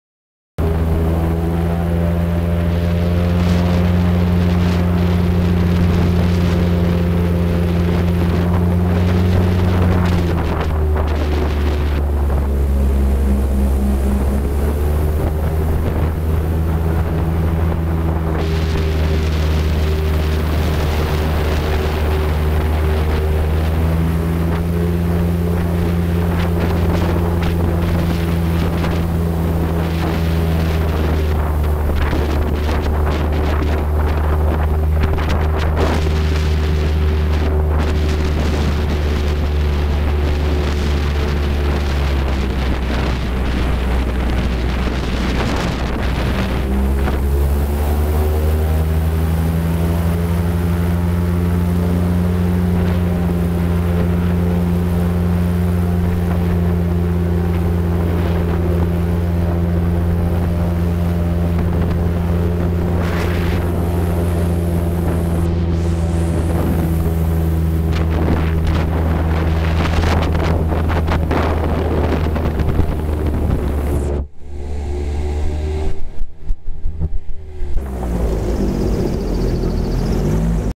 emoción en los Everglades, Florida
un paseo sensacional en los pantanos de Florida infestados de cocodrilos y lagartos